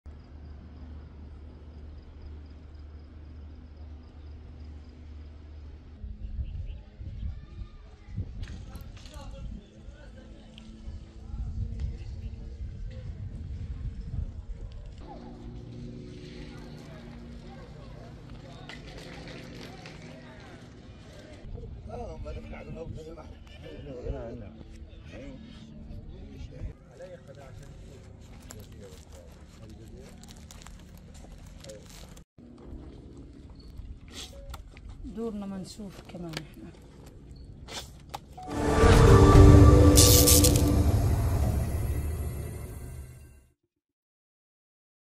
دمار كبير في أبراج الشيخ زايد شمال قطاع غزة
دُمرت بالكامل.. هكذا أصبحت أبراج الشيخ زايد شمال قطاع غزة والتي كانت تضم أكثر من 700 أسرة فلسطينية اضطروا للنزوح والعيش في الخيام.